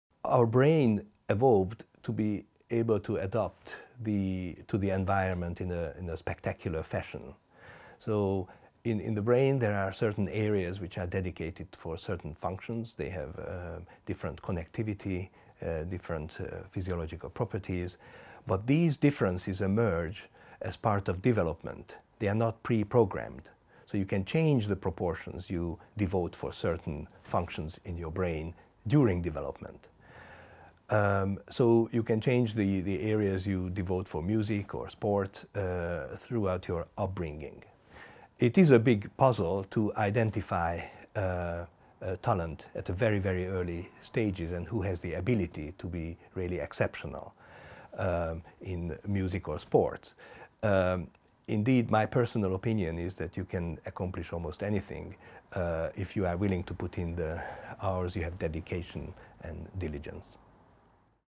Can anyone become a concert pianist or pro tennis player?
DescriptionProfessor Zoltán Molnár from the University of Oxford.

This video was recorded for the Brain Diaries exhibition at the Oxford University Museum of Natural History, 10 March 2017 - 1 January 2018.